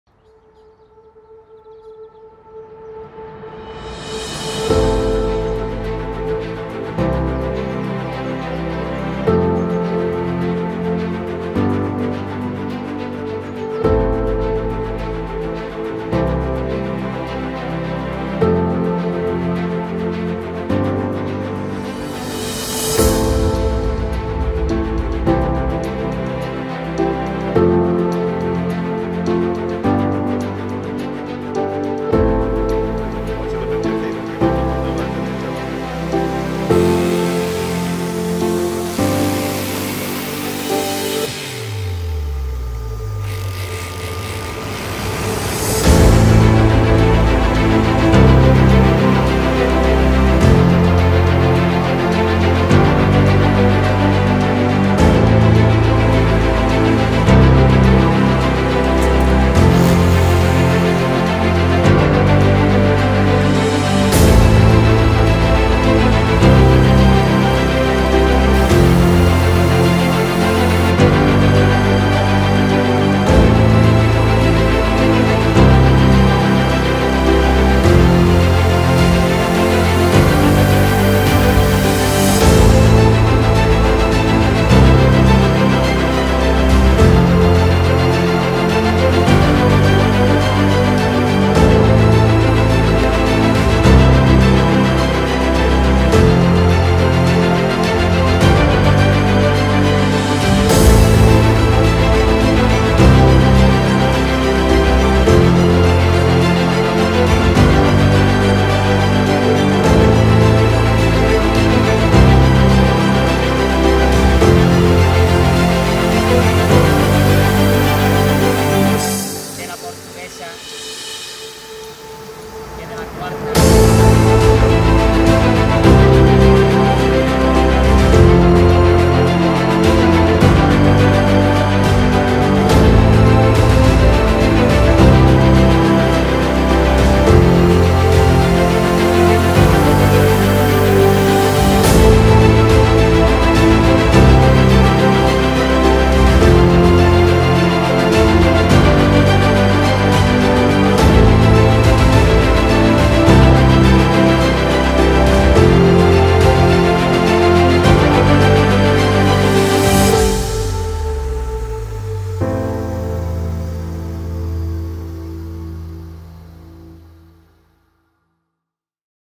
Rehabilitación piscina con lamina armada (high definition 4k)
Rehabilitación e impermeabilización de piscinas con lamina armada

Watch how we install reinforced PVC membranes.
Instalación de láminas armadas en una piscina.